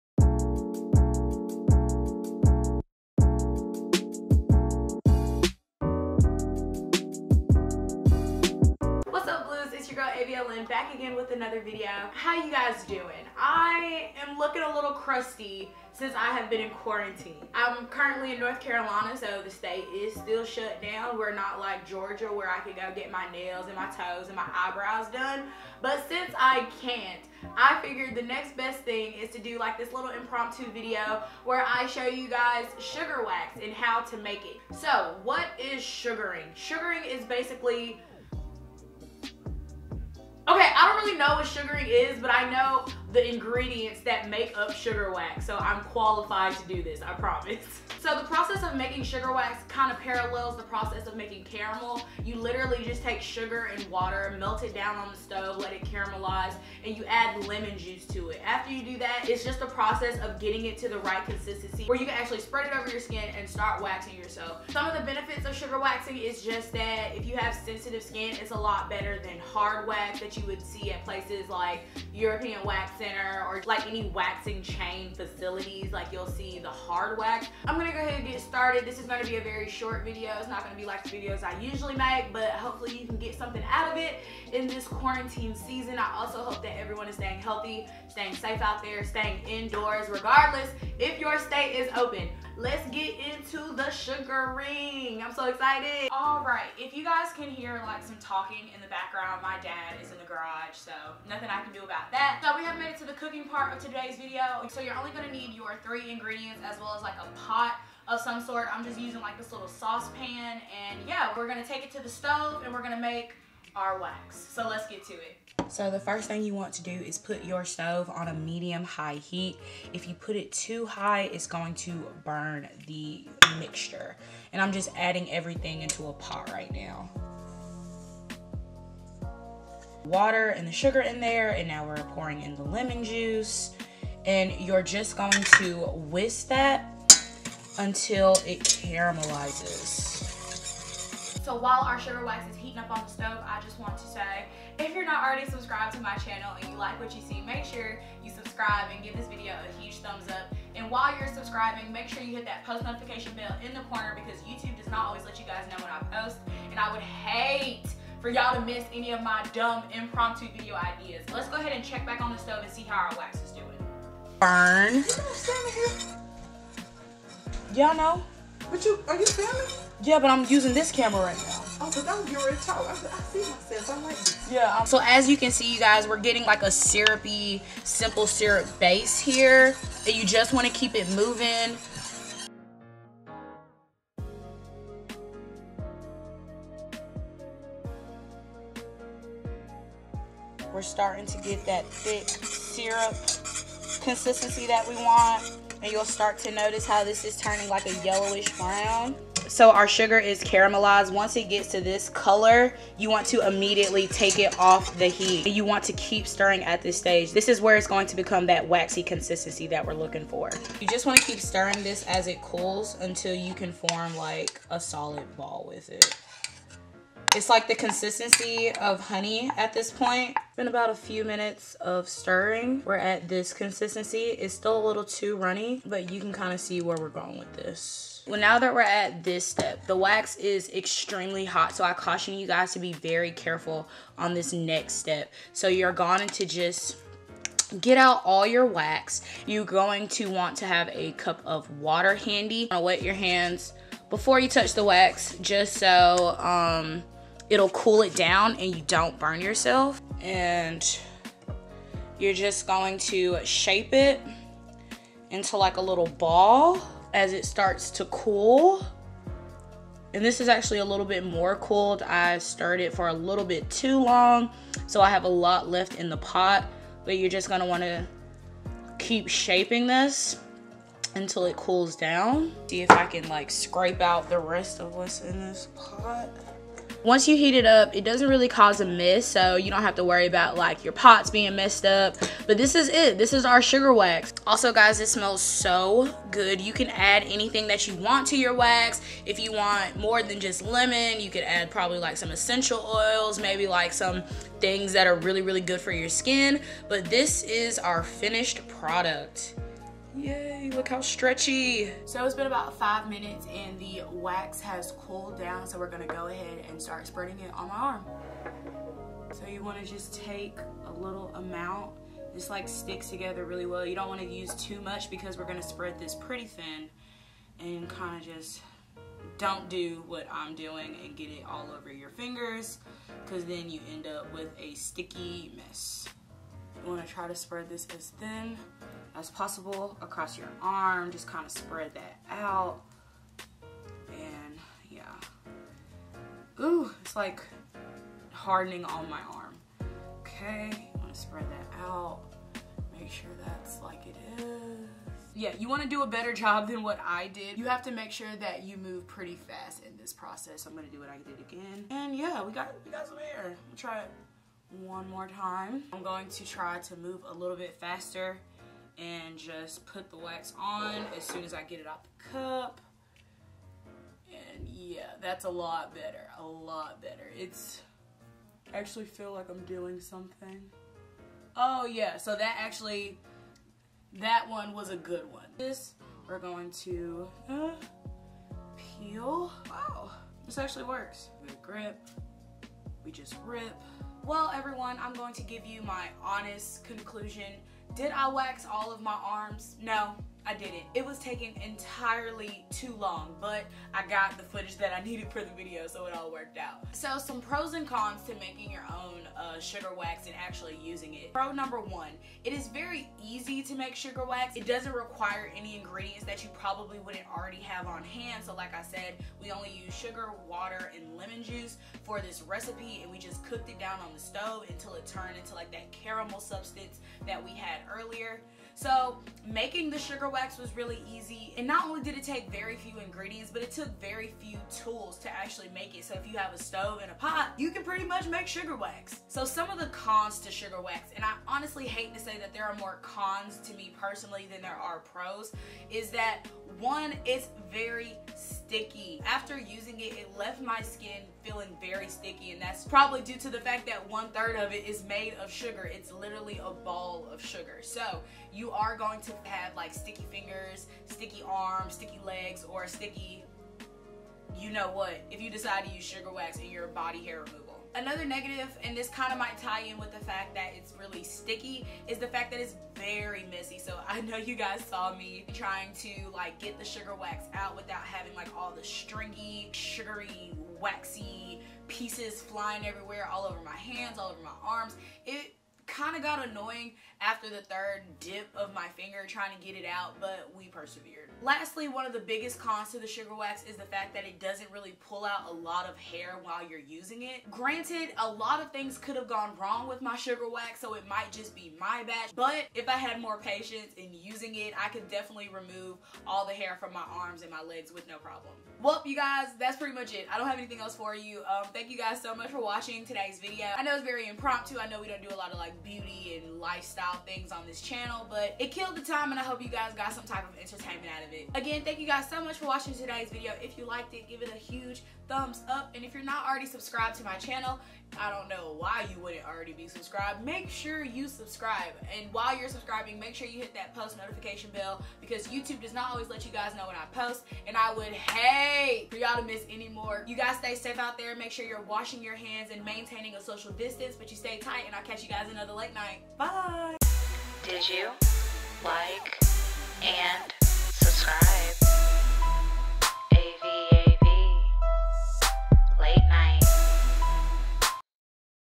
Making DIY Sugar Wax + Sugar Waxing Myself! | European Wax Center is closed :(
Wassup Blues!! This week I made my very own DIYsugarwax ! If I can't go to European Wax Center well f*ck it IT'S COMING TO ME! (kinda) Ladies if you ever wanted to learn how to master DIYSugaring WATCH THIS VIDEO!

You guys stay tight and I'll see you another late night BYEEEE!